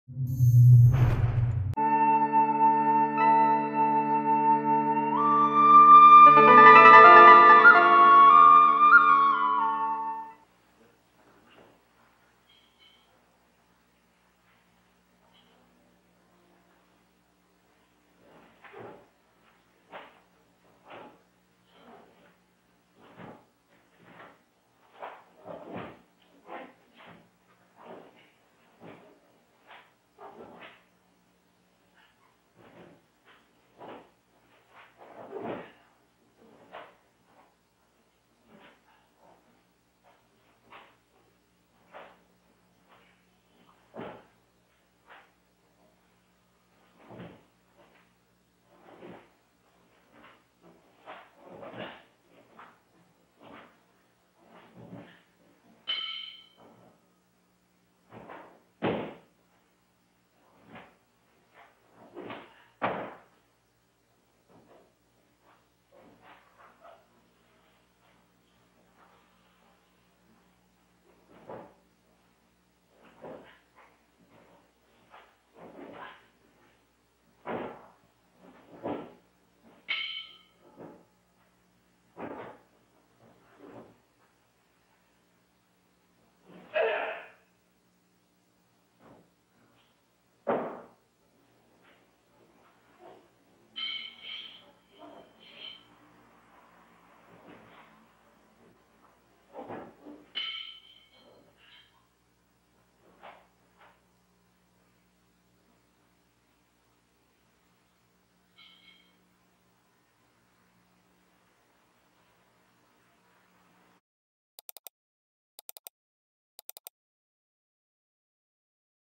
Tokashiki Mamoro Chatan Yara No Sai
Chatan Yara No Sai is demonstrated by Tokashiki Mamoro on Okinawa.